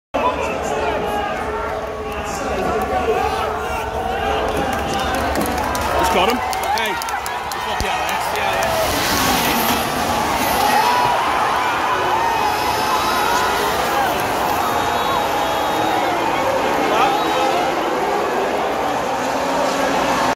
Overtake Max Verstappen VS Lewis hamilton | Max Verstappen Formula 1 World champion Winner
Max verstappen Formula 1 world champion 2021 winning overtake.

Max verstappen berhasil menjadi Juara dunia Formula 1 musim 2021 melalui drama di lap lap akhir. Max verstappen melakukan overtake terhadap lewis hamilton di tikungan ke 5 sirkuit Yas marina,Abu dhabi di lap terakhir setelah sempat diwarnai dengan keluarnya safety car. Max verstapen tidak terkejar oleh Lewis hamilton setelahnya,dan berhasil menyentuh garis finish pertama untuk mengamankan dan menggondol titel Juara dunia Formula 1 musim 2021.